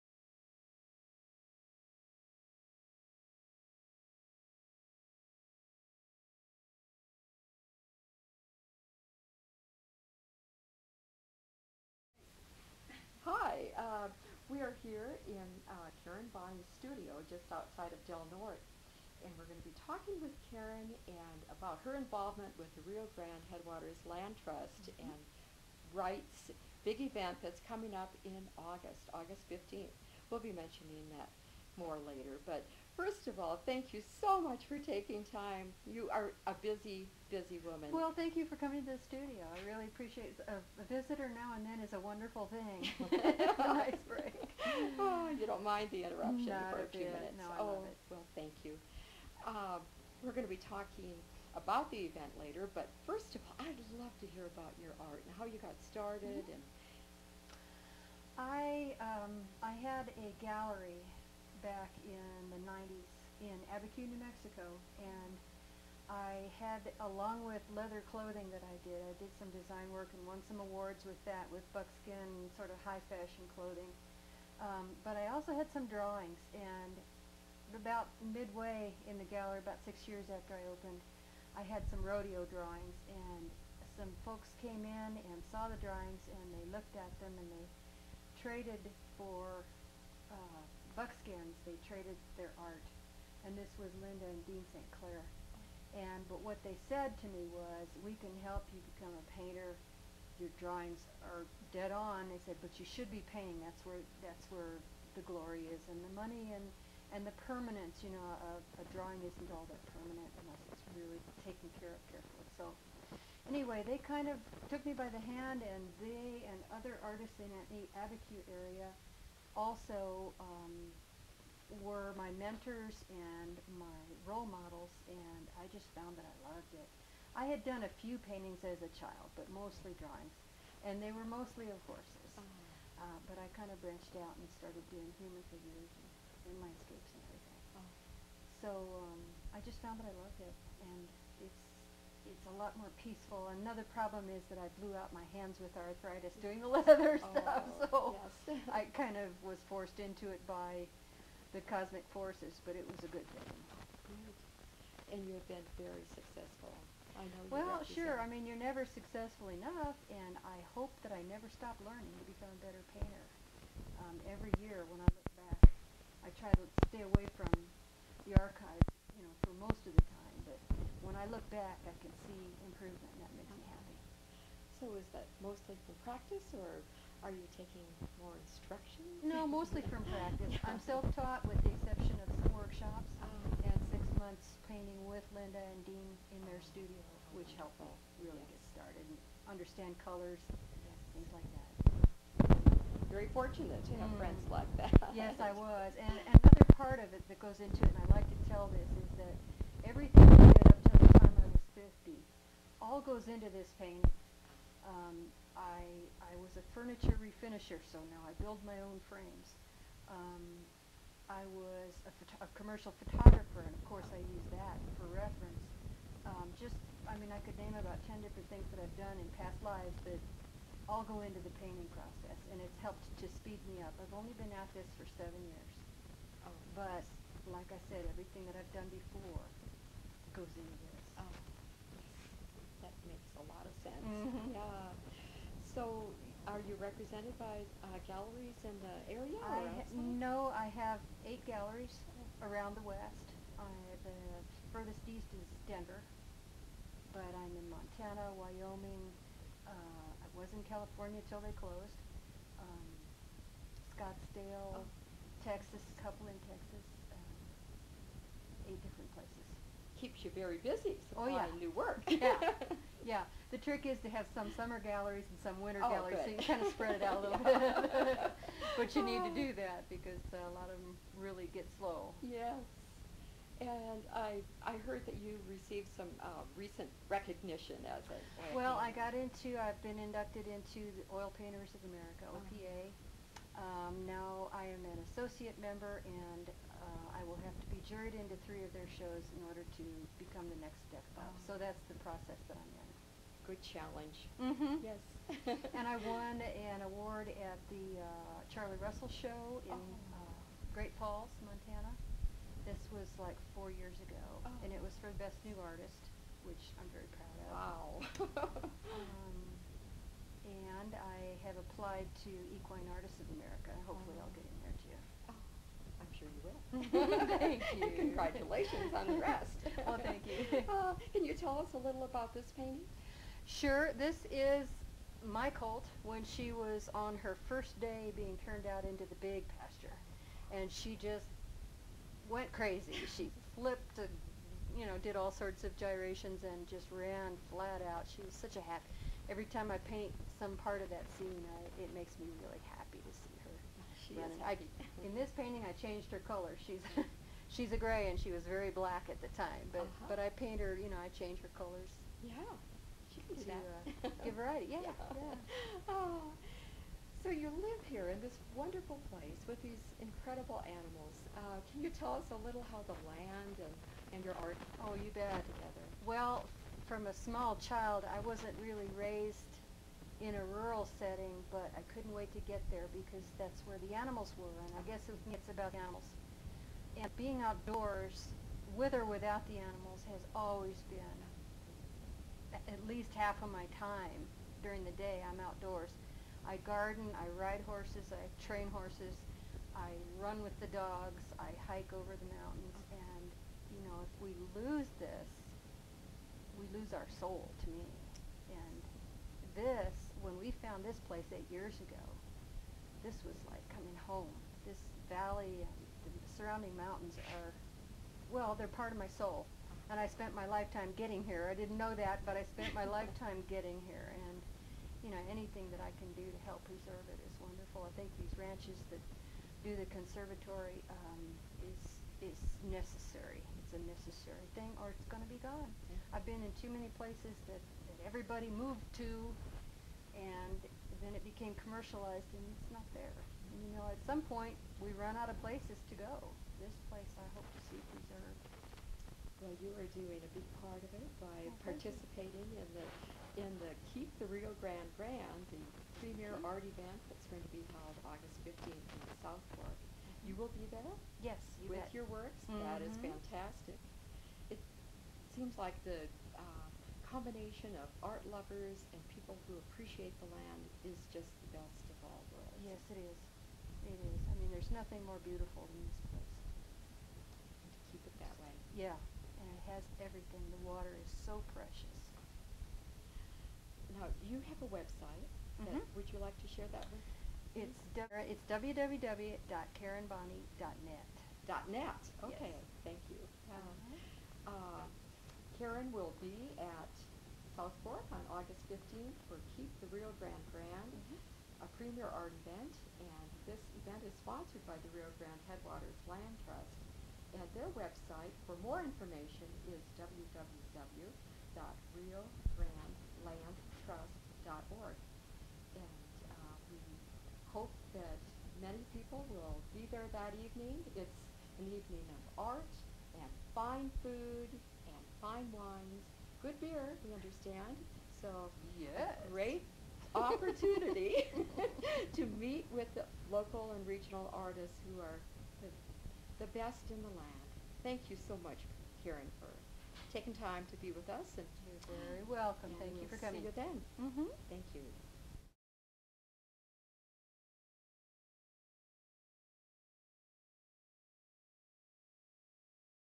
Keep the Rio Grande Grande - Interview with Painter Karen Bonnie
The premier art event of the season, Keep the Rio Grande Grand is a fine art sale and show to benefit the conservation work of the Rio Grande Headwaters Land Trust. of Del Norte, Colorado, discuss her work and commitment to conservation. Join us and 28 participating artists at the South Fork Community Center in South Fork, Colorado for an evening of fine art, fine wine, great food and beer at the 2nd Annual Keep the Rio Grande Grand on August 15, 2009. for full information.